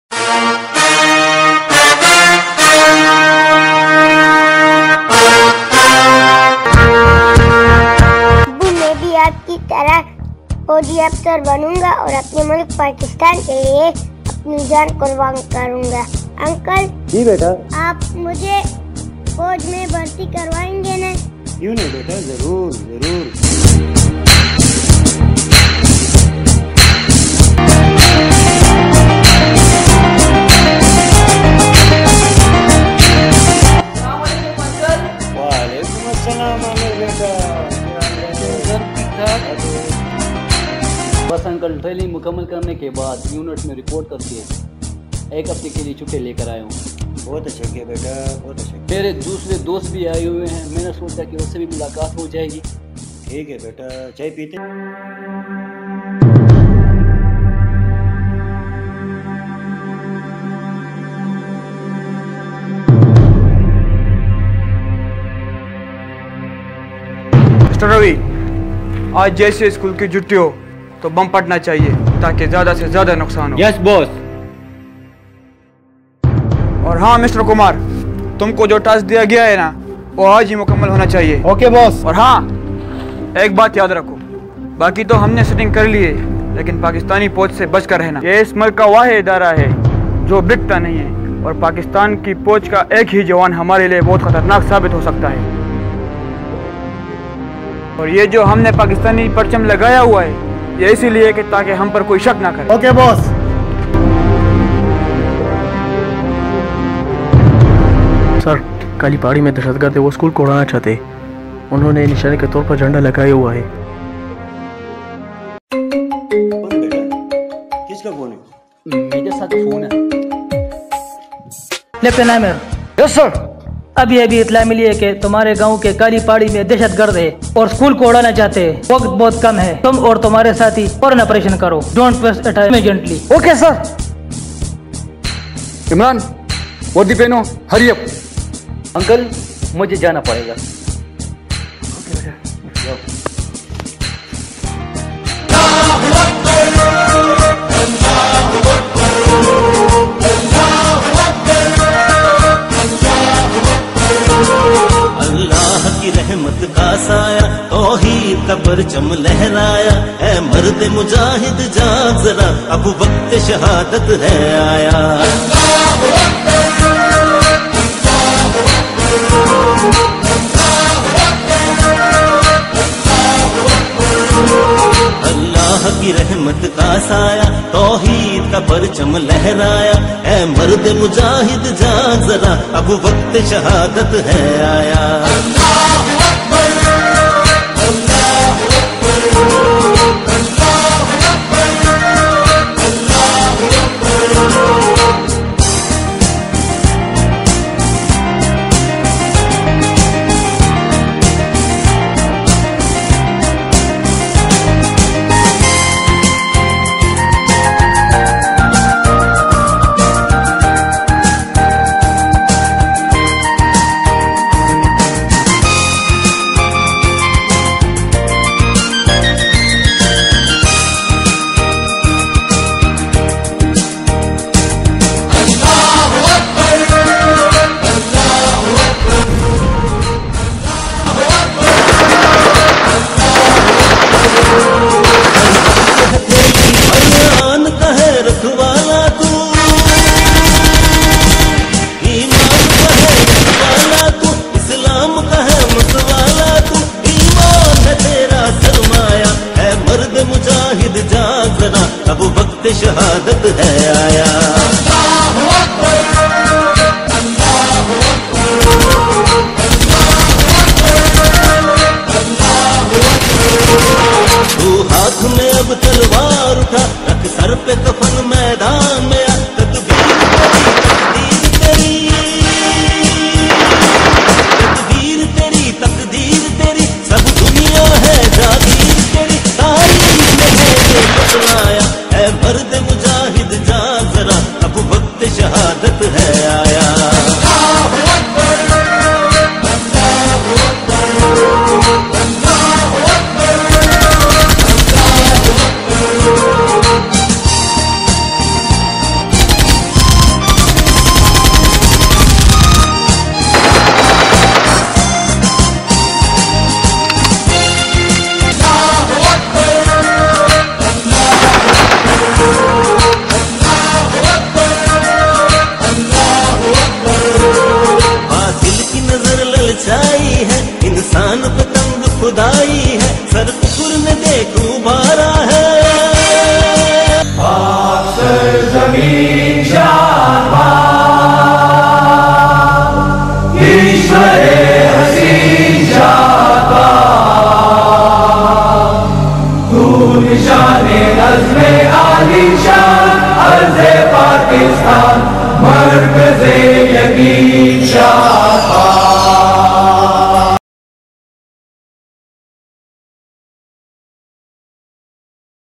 Pak Army Mission | Army Jazba | Anbar Vines | 2022
Pak Army ki qurbani.
army ka jazba aisa hota hai
Jis Tarah is video mein dikhaya gaya hai
yah Apne Mulk ke liye larte hain
aur Hamara hapzat bhi karta hai
to doston Hamara Facebook page hue hain
to use bhi follow bi Kare